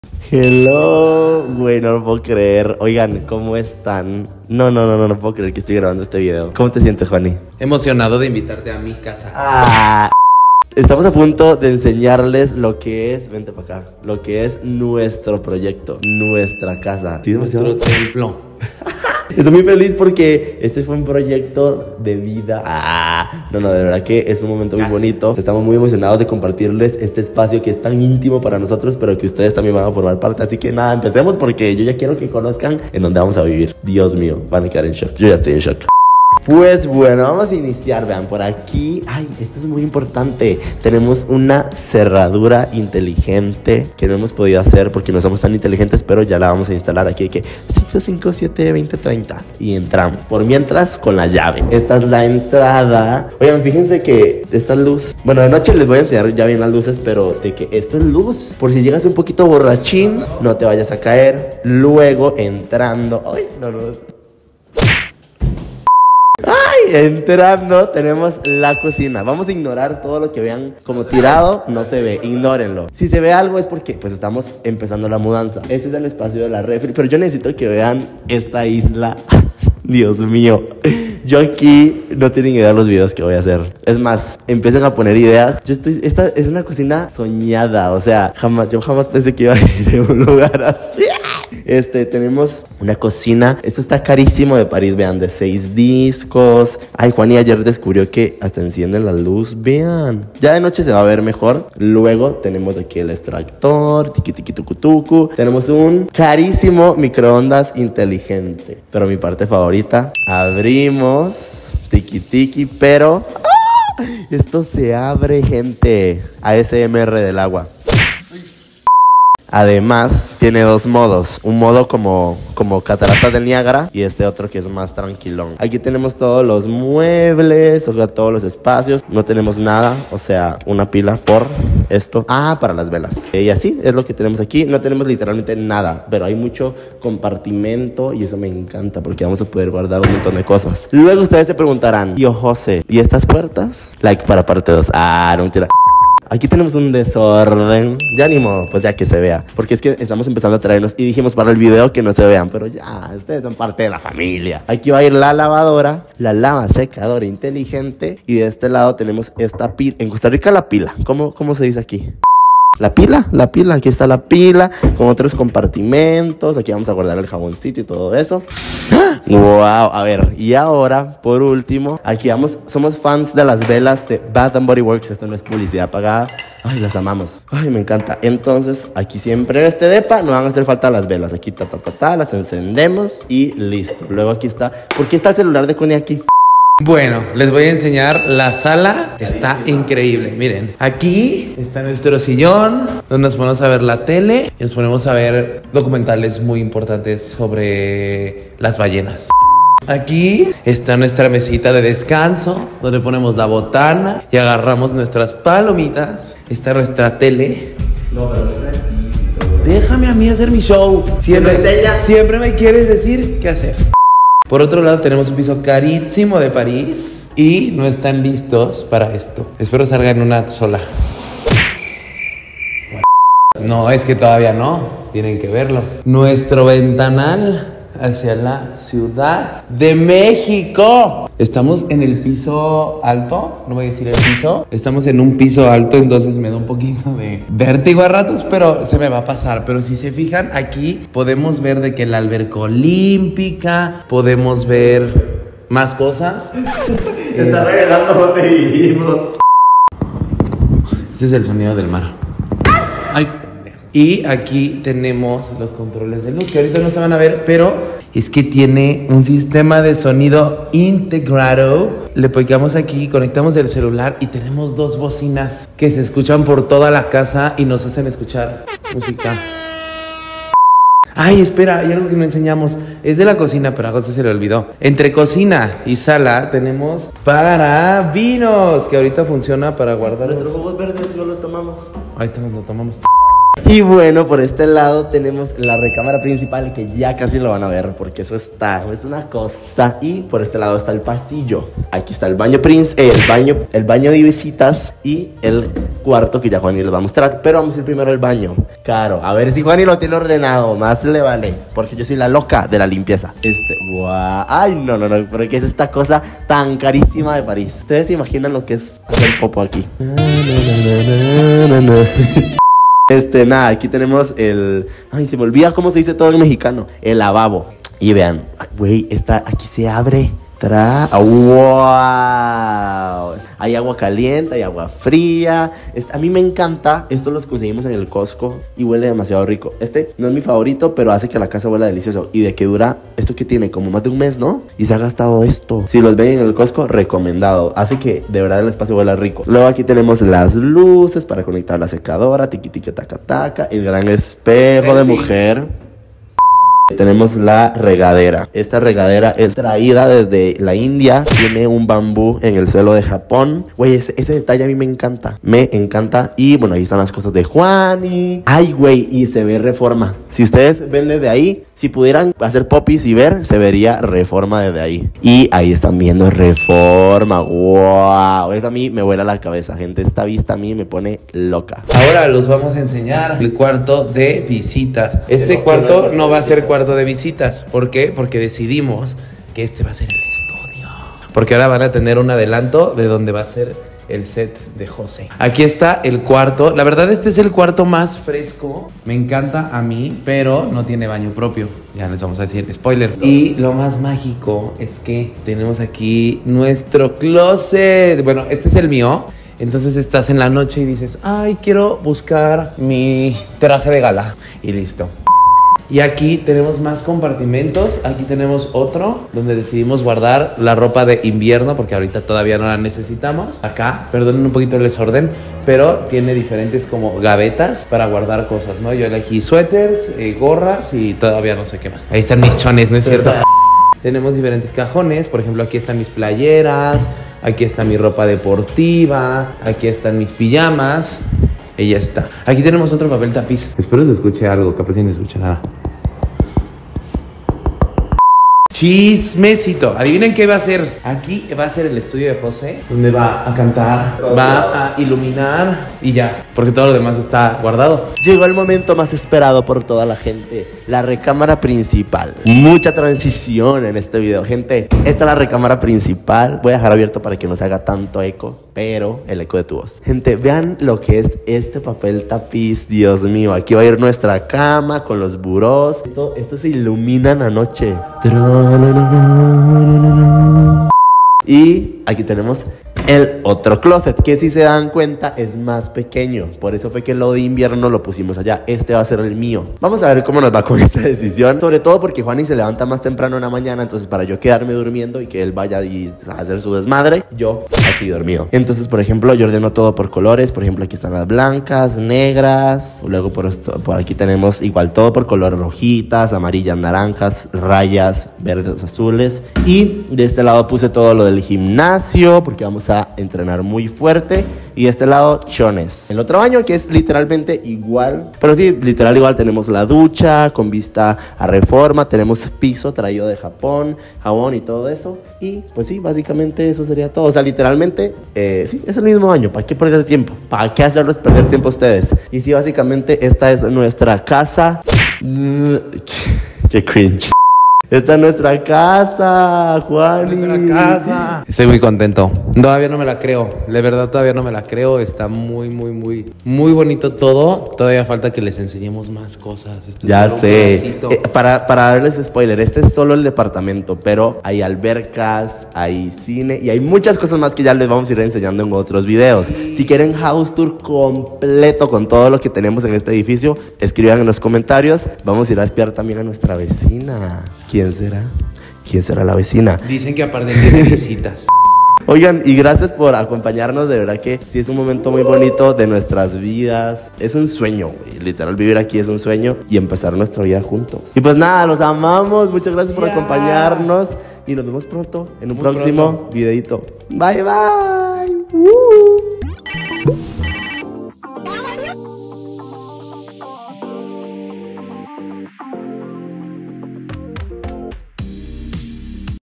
TOUR POR MI NUEVO DEPARTAMENTO EN CDMX ¿NOS MUDAMOS JUNTOS?
HOUSE TOUR POR MI NUEVO DEPARTAMENTO EN CDMX *EN LAS ALTURAS*

Familia que emoción ❤️ junto con Juani estamos muy felices de compartirles nuestro nuevo espacio donde seguimos escribiendo una historia hermosa. Estamos en un nuevo departamento donde viven otros youtubers 😱 aquí va el House Tour